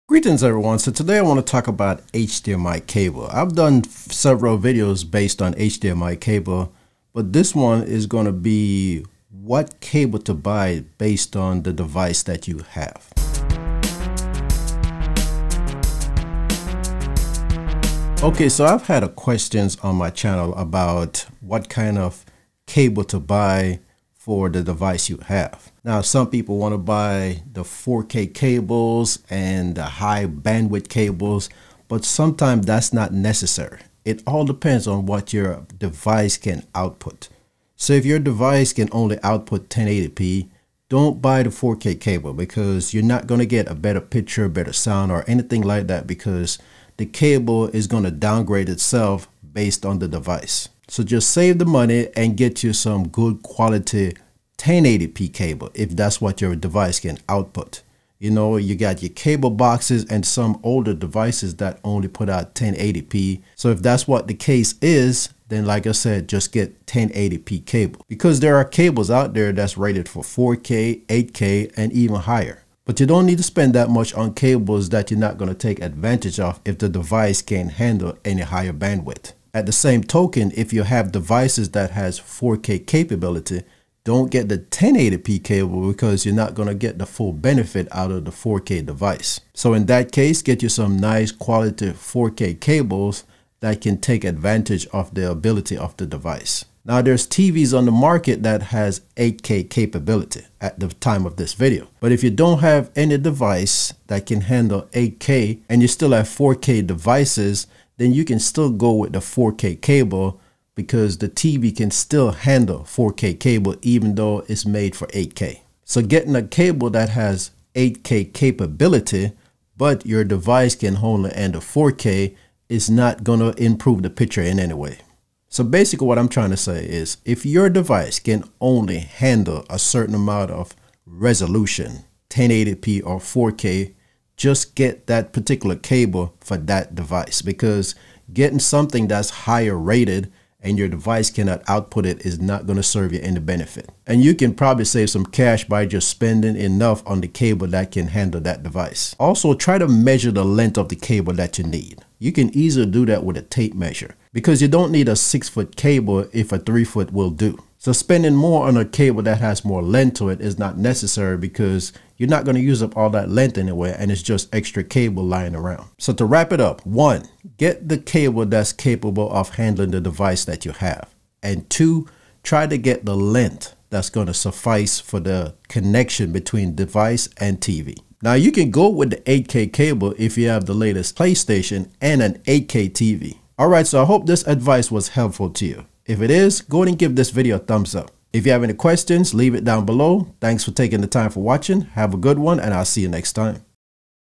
Choose The Right HDMI Cable For Your Devices
Choosing the right HDMI cable can be confusing for you to figure out, 1080P, 4K, or 8K, which should you choose?  Well, It all depends on the device output capability, most older devices can only handle 1080P output, newer devices can support 4K and a handful can only handle 8K.

Knowing that you only need the right cable that each device can support. Let me help you decide how to choose the right HDMI cable for your devices, and possibly save some money.